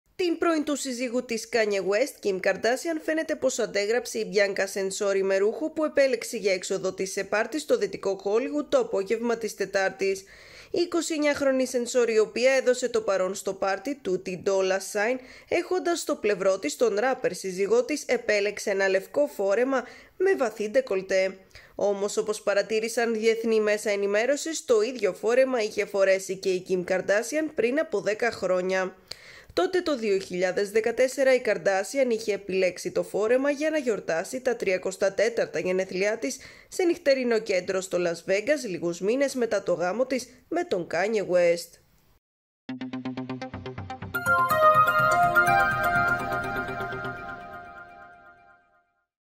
Αντέγραψε την πρώην: H Μπιάνκα Σενσόρι εμφανίστηκε με σέξι φόρεμα που είχε φορέσει η Κιμ Καρντάσιαν
Αντέγραψε την πρώην: H Μπιάνκα Σενσόρι εμφανίστηκε με σέξι φόρεμα που είχε φορέσει η Κιμ Καρντάσιαν | Pronews TV